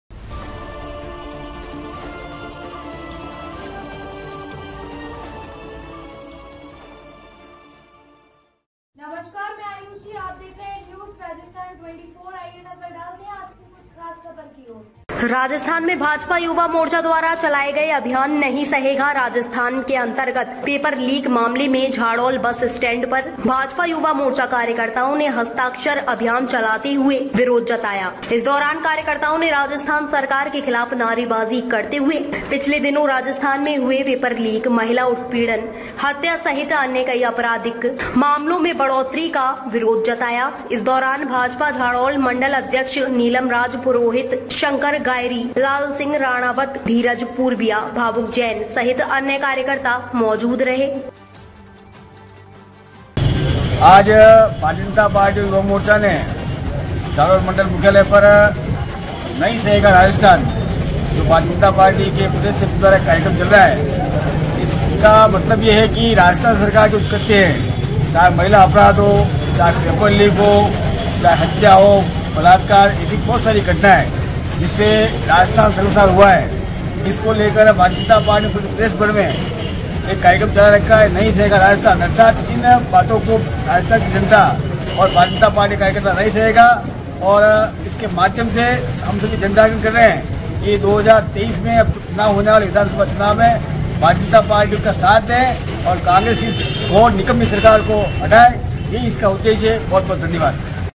jhadol राजस्थान सरकार के खिलाफ भाजपा युवा मोर्चा ने झाड़ोल में चलाया हस्ताक्षर अभियान
राजस्थान सरकार के खिलाफ भाजपा युवा मोर्चा ने झाड़ोल में चलाया हस्ताक्षर अभियान